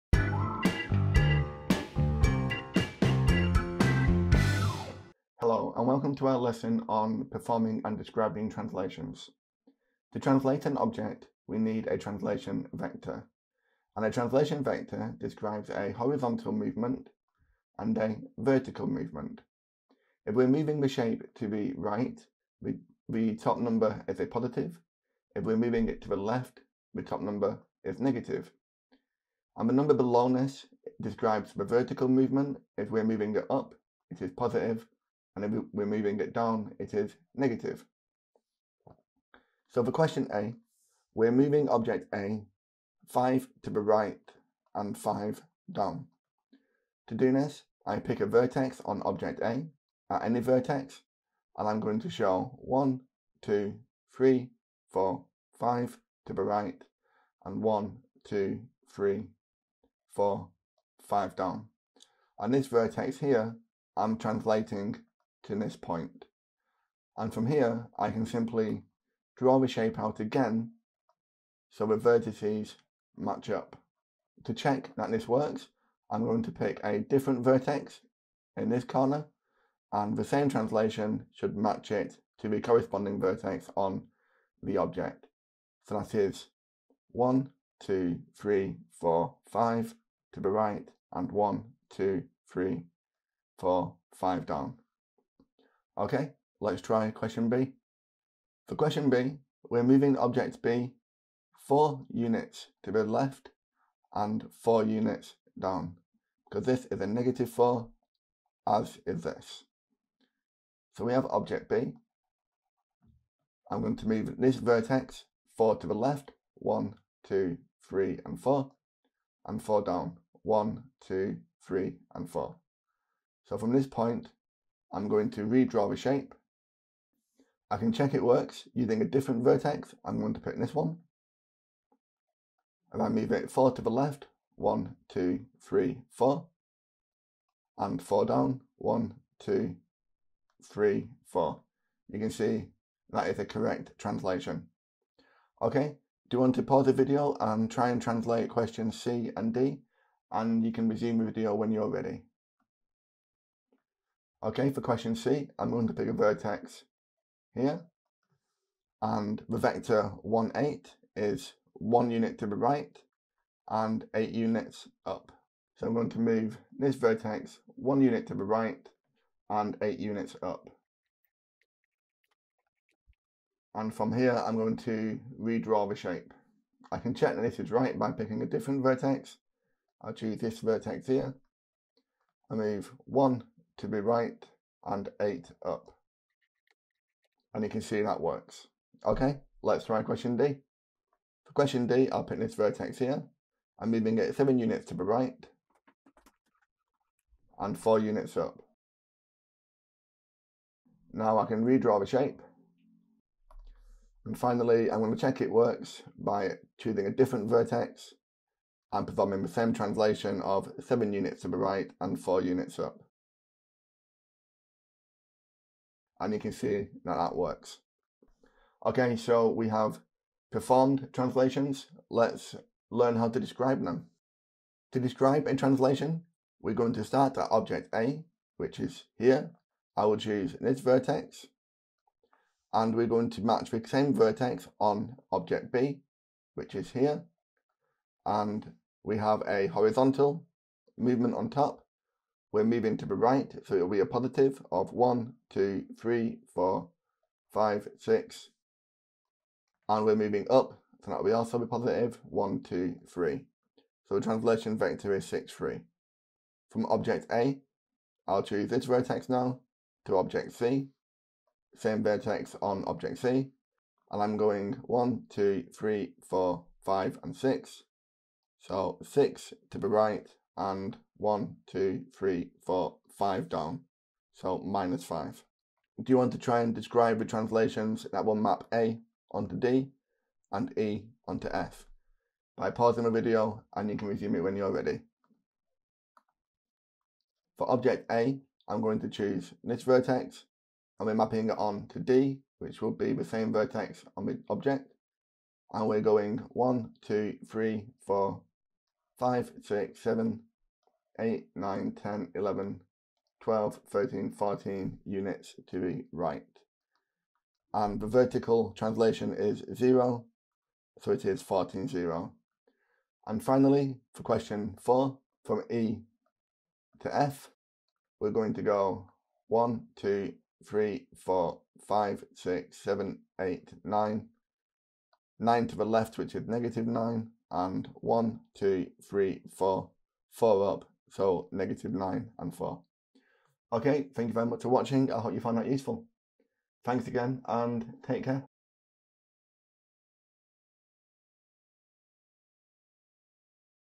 Performing and Describing Translations
How to perform and describe a translation using column vectors

The full lesson includes a starter, 2 mains, plenary and a worksheet.  Members of Mr Mathematics can download this